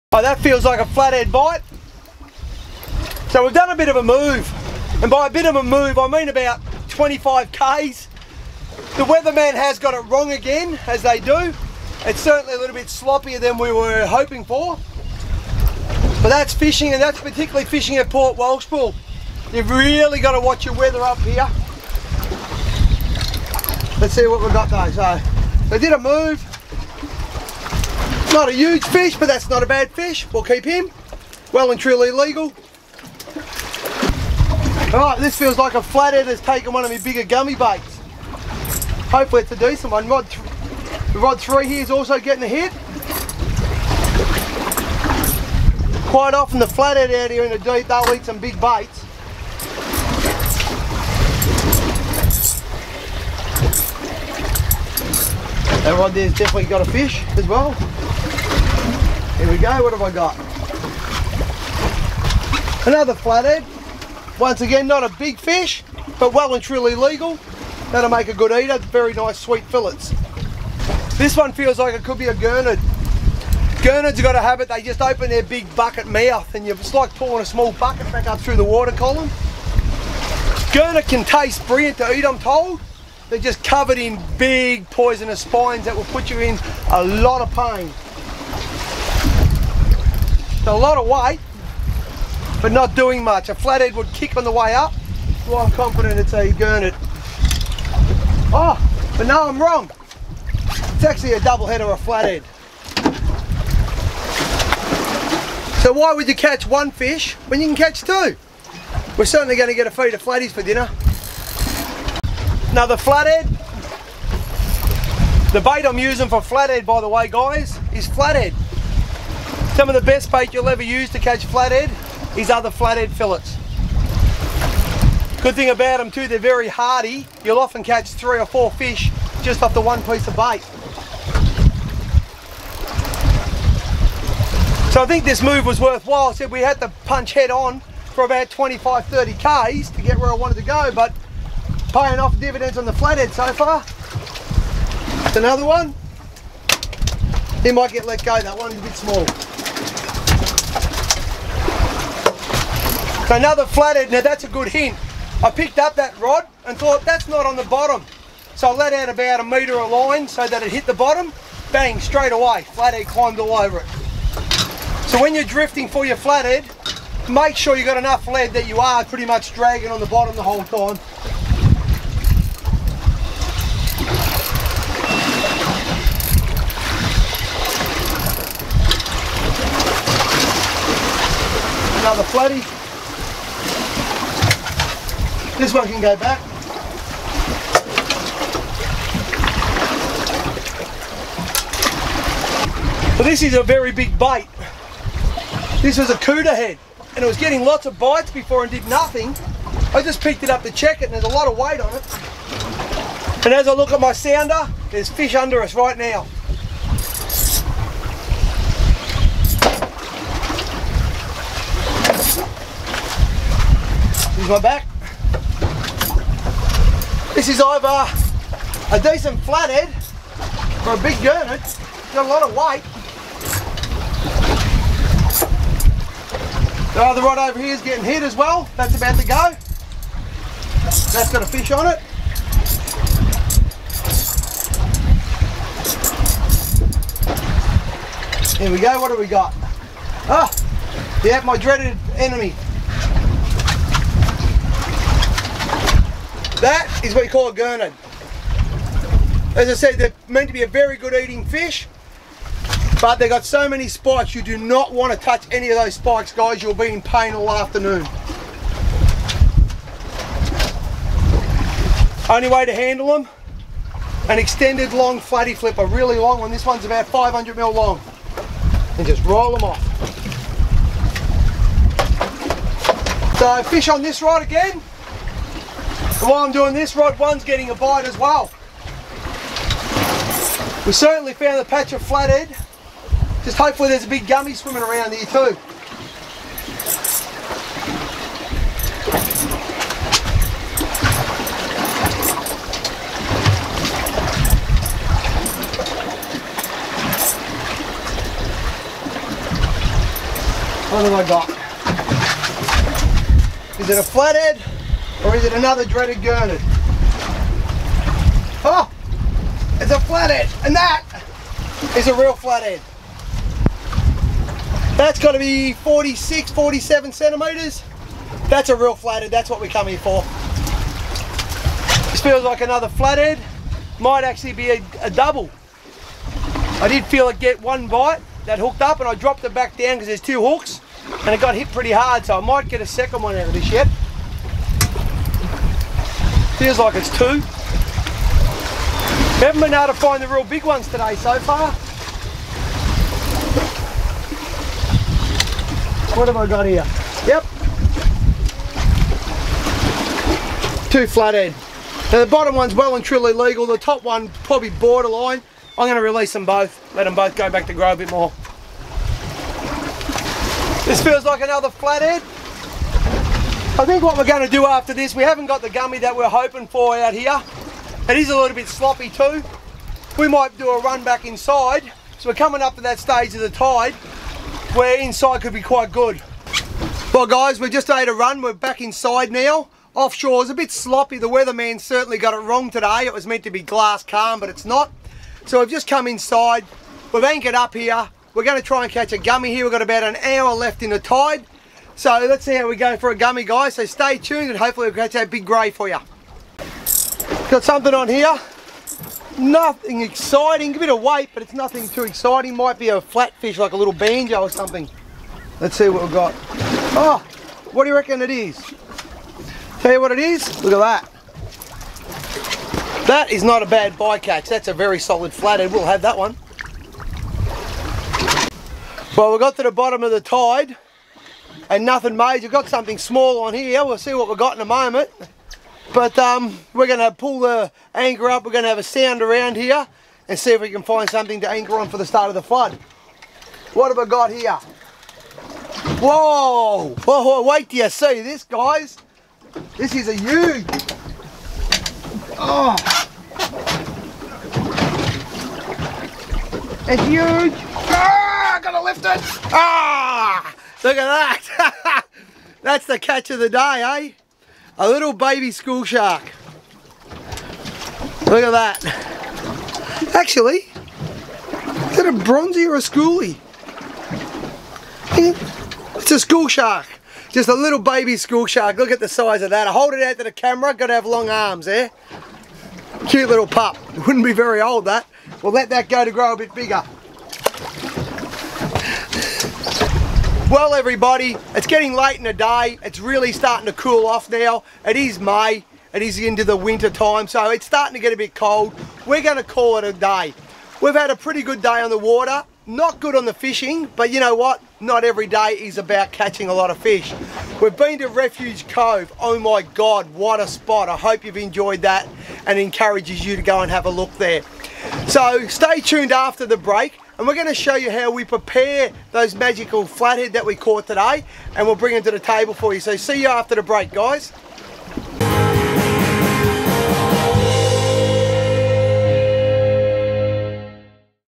Fishin with Fergy Refuge Cove Ep9 Pt2
Today we venture to the beautiful Refuge Cove in Wilson's Prom. We hope you enjoy the show